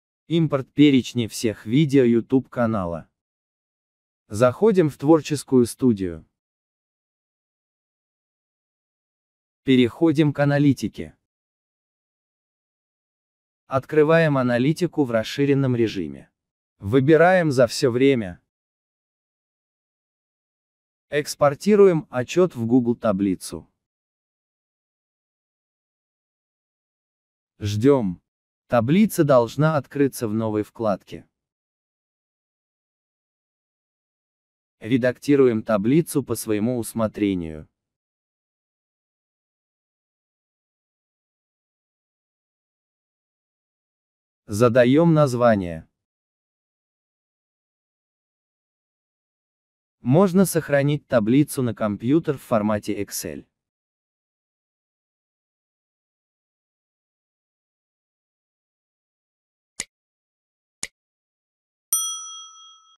импорт перечня всех видео ютуб канала в таблицу за 1 минуту
Импортируем перечень всех видео на своем канале в Google таблицу и затем в Excel.
Перед импортом можно так же настроить отчет по своему усмотрению. Об этом в этом видео не рассказываю.
Мои плейлисты:
1️⃣ Видеоуроки - быстро, просто, результативно